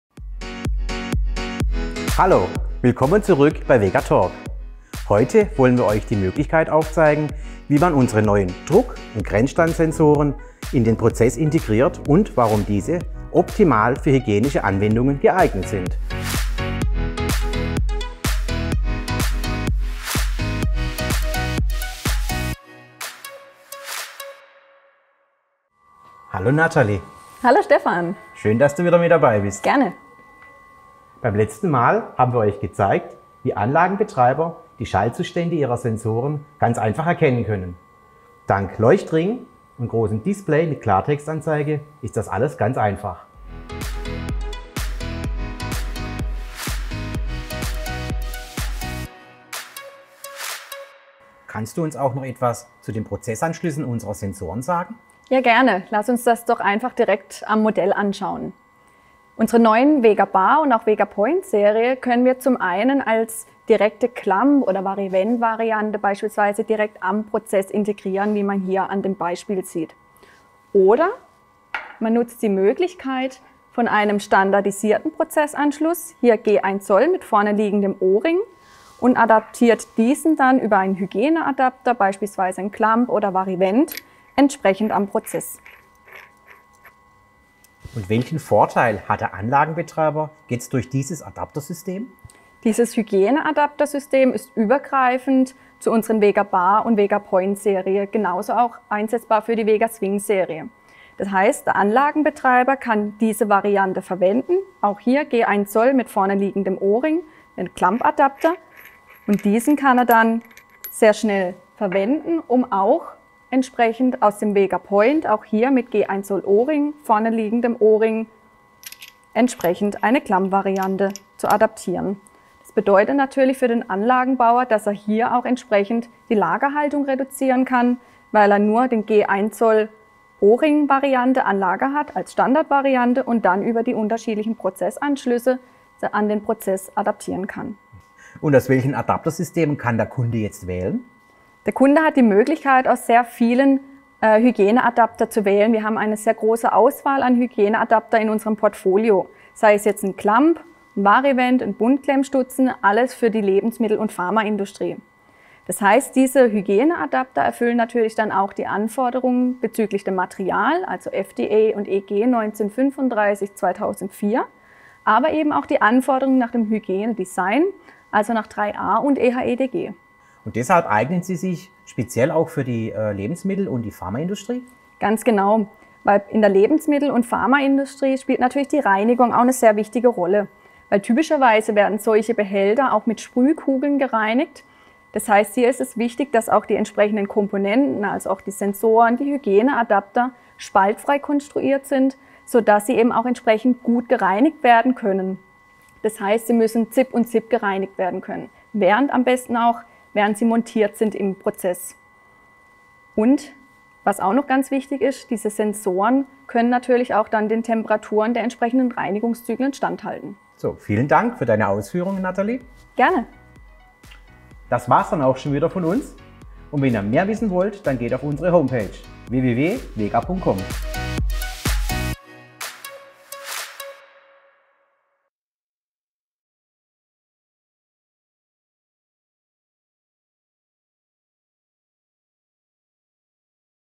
🔩 Wie können Sensoren in hygienische Prozesse integriert werden? | VEGA talk
Für Sensoren zur Messung von Füllstand und Druck sowie Grenzschalter bietet VEGA ein einheitliches Hygieneadapterkonzept, das EG 1935/2004-, FDA-, EHEDG-, 3-A-konform ist. Ob metallisch dichtend oder mit O-Ring-Dichtung: Die Sensoren passen für alle handelsüblichen hygienischen Anschlüsse und sind somit optimal geeignet für den Einsatz in der Lebenmittelproduktion und der pharmazeutischen Industrie.

Vorteile der einheitlichen Hygieneadapter:
✅ Flexible und einfache Integration
✅ Schneller Austausch
✅ Reduzierte Stillstandzeiten
✅ Weniger Varianten an Lager

Noch mehr Experten-Tipps und Neuheiten rund um Füllstand- und Druckmesstechnik?